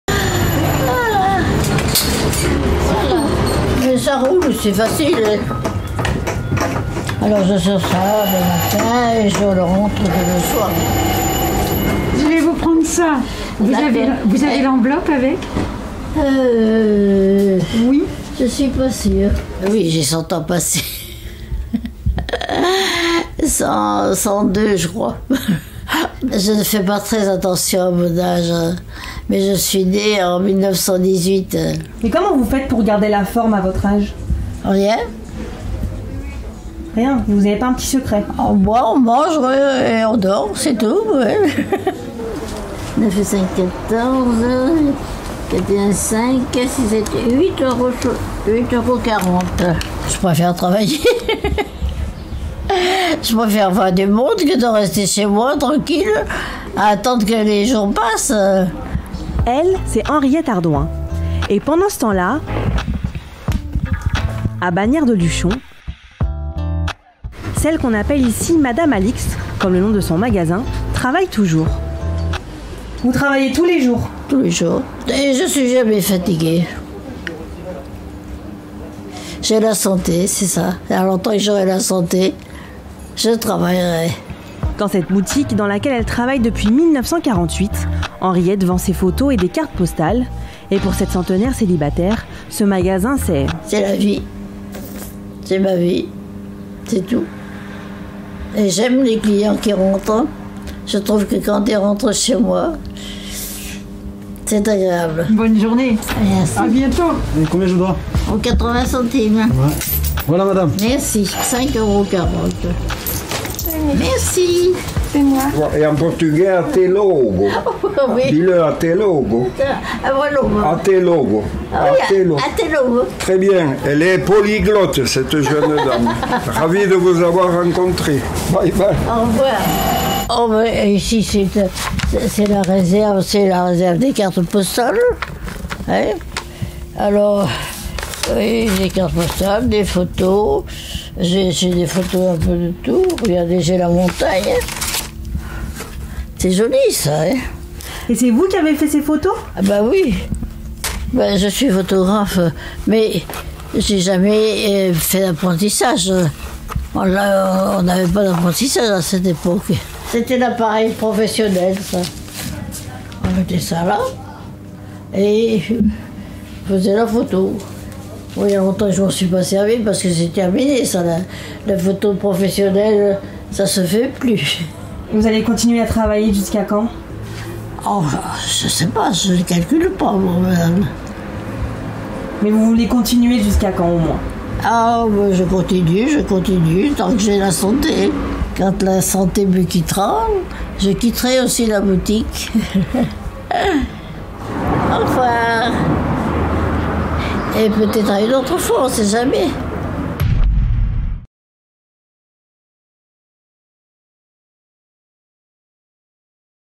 À 102 ans, Henriette travaille toujours dans sa boutique ouverte en 1948
Henriette a 102 ans. Et pendant ce temps-là, elle travaille toujours dans sa boutique de photos et cartes postales ouverte en 1948 à Luchon dans les Pyrénées…